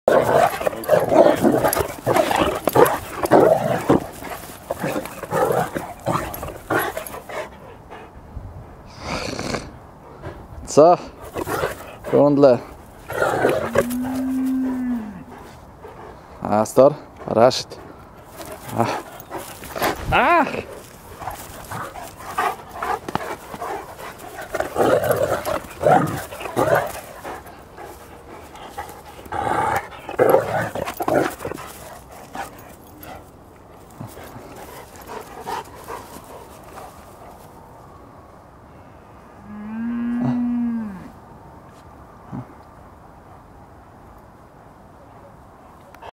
Astor & Rashid #2 (zabawy koni)
Cane Corso Italiano (7m.o.) + Rhodesian Ridgeback (5y.o.)
"Horses' plays"